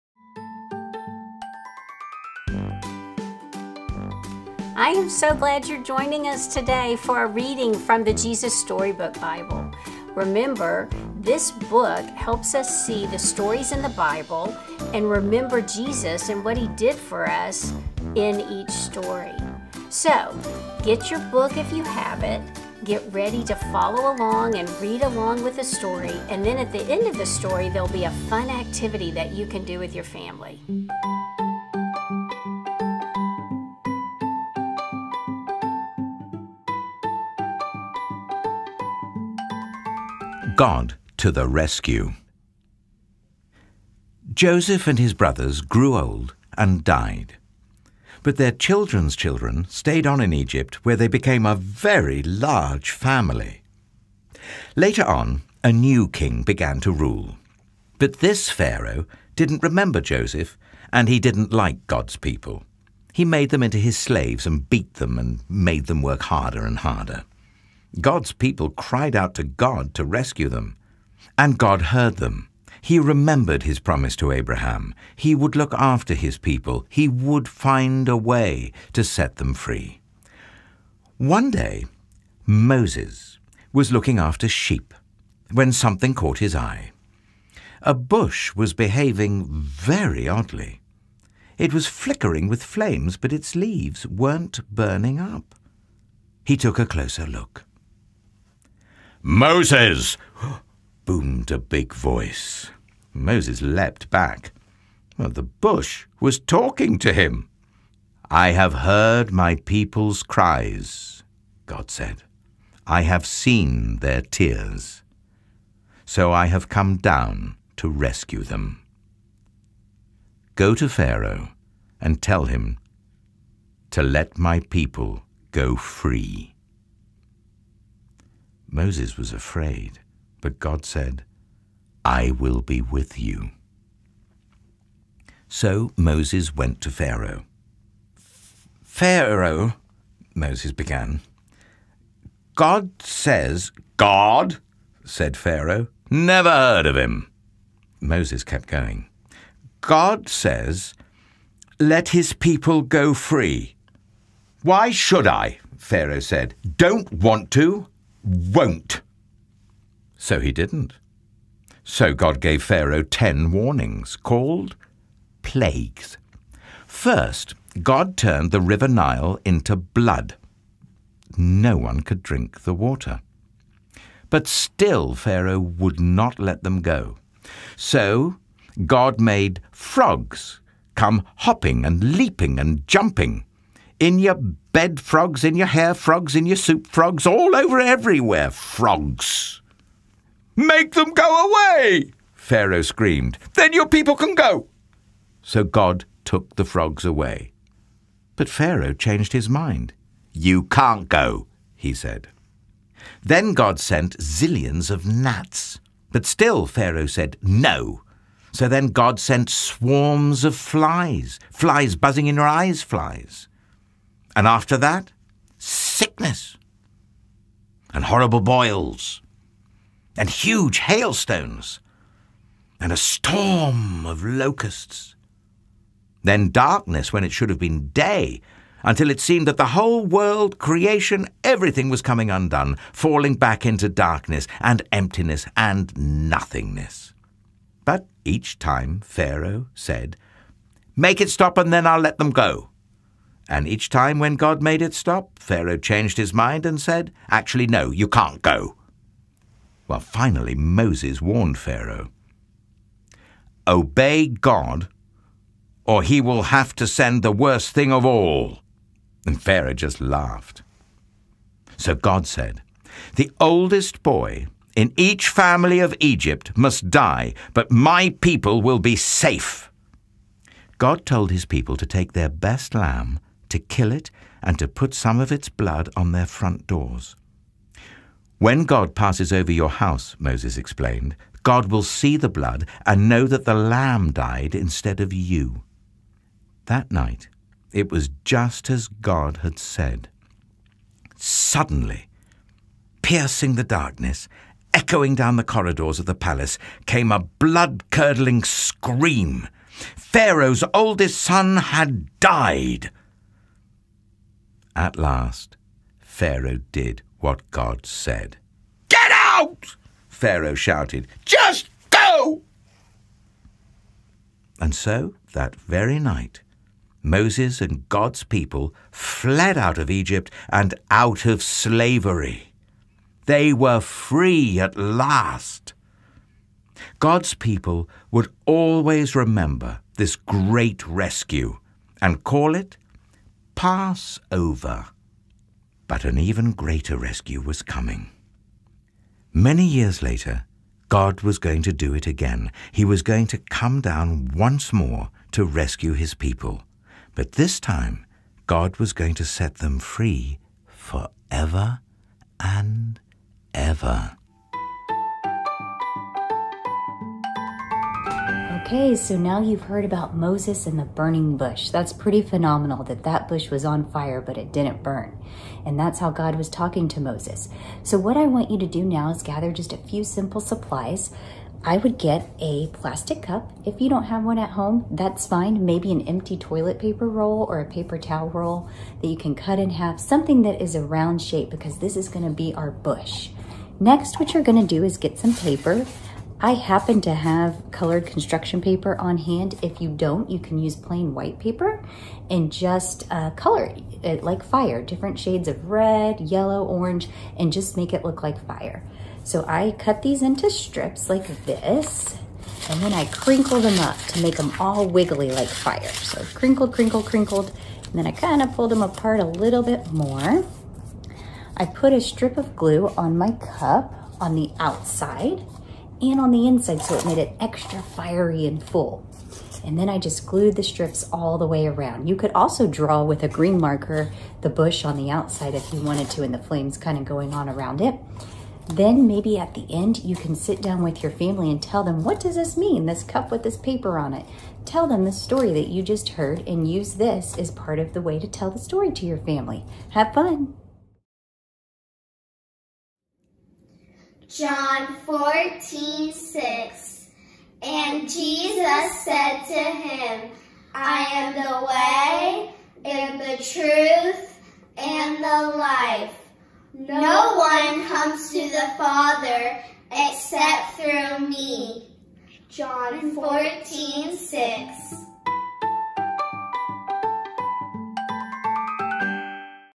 Part 6 Jesus Storybook Bible: God to the Rescue
God to the Rescue can be found on pages 84-91 in the Jesus Storybook Bible. This story talks about Moses and the Israelites escaping from Egypt (taken from Exodus 3-13).

The supplies needed for the activity include a plastic cup/empty paper towel roll, orange/red/yellow tissue paper, white paper in strips, markers, glue/tape, and scissors.